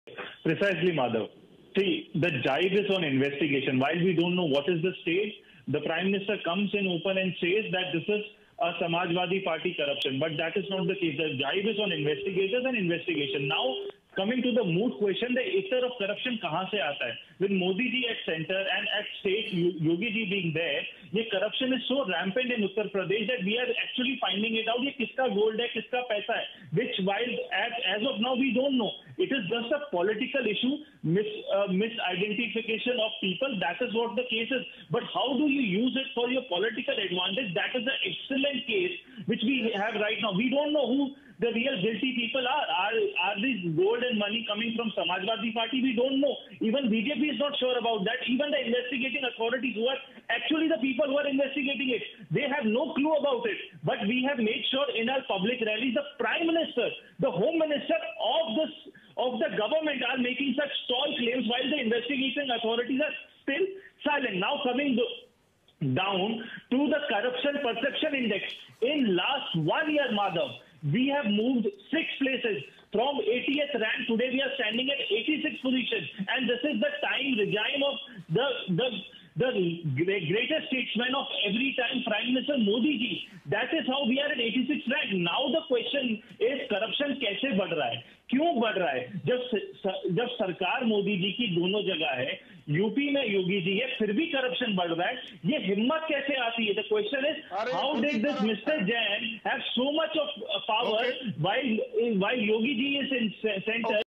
Manasvi Thapar on Times Now (28.12.2021)
Even while investigation agencies are not sure who is linked to this case, leaders like Modi and Shah are making tall claims ... In the last 1 year, India has moved 6 places (from 80 to 86) in Corruption Perception Index:

Ref - Times Now